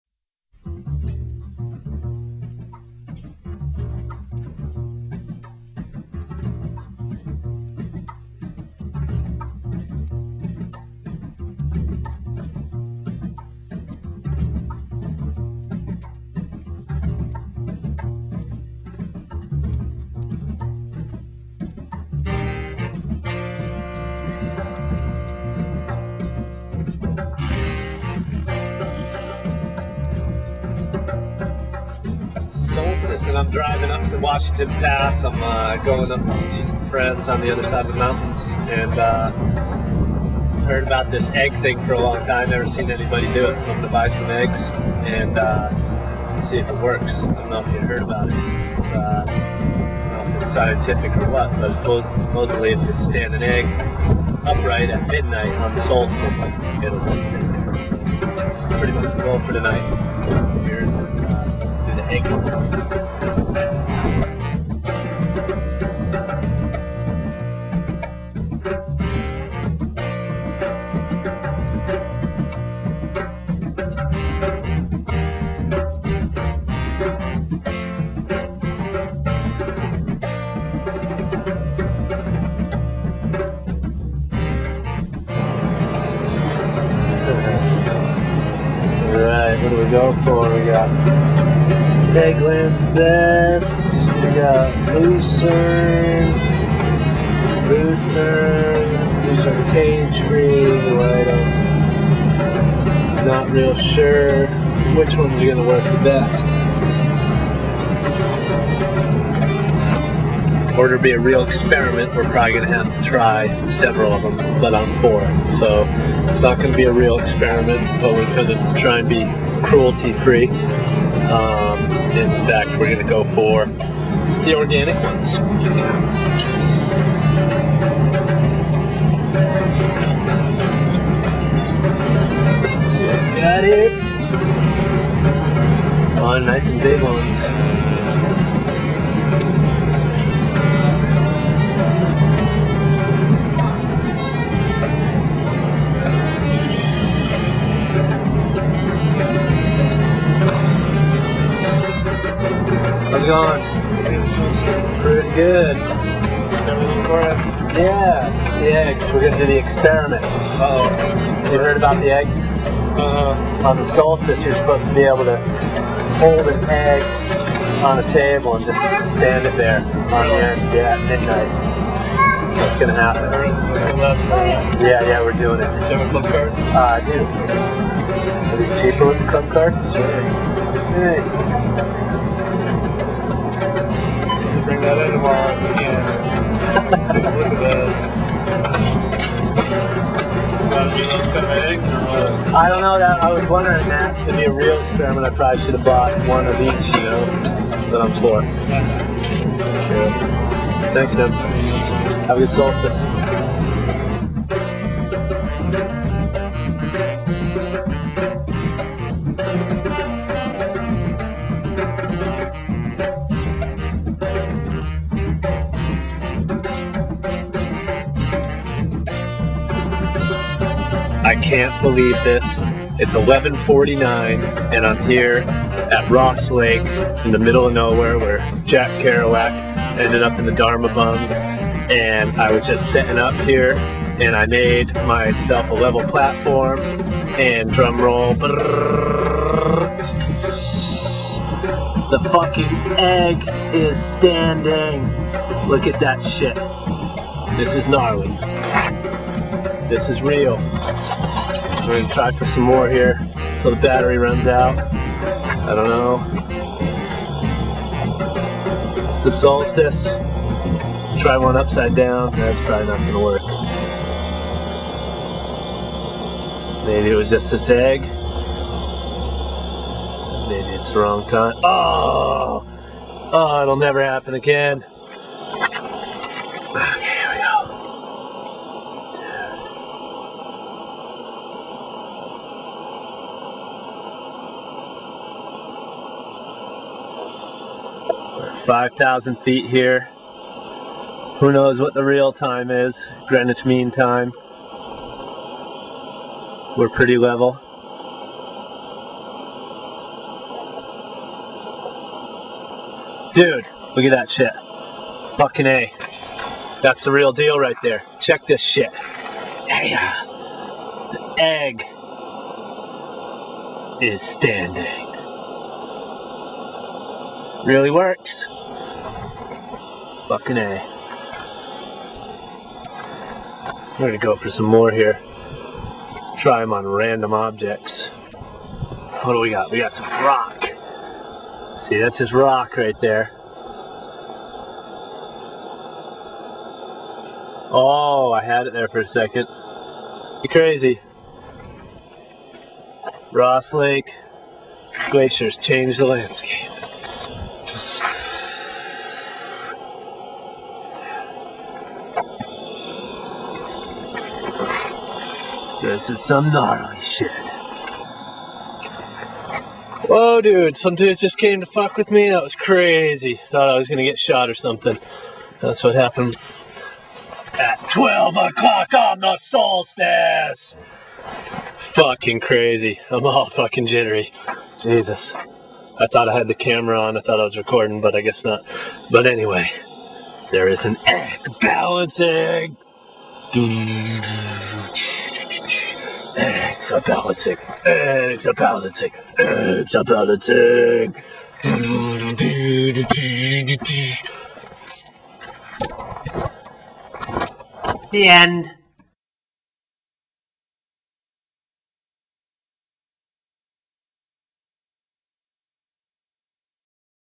solstice egg stand
balancing an egg on the solstice, super duper wicked sick shit!